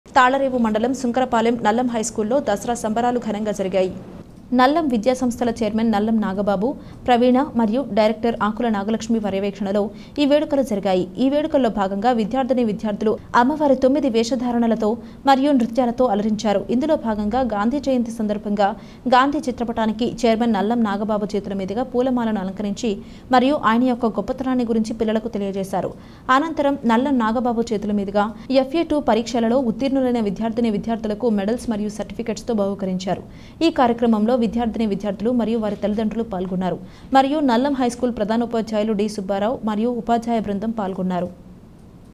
సుంకరపాలెం నల్లం హైస్కూల్లో ఘనంగా దసరా సంబరాలు
తాళ్ళరేవు మండలం సుంకరపాలెం నల్లం హైస్కూల్ లో దసరా సంబరాలు ఘనంగా జరిగాయి.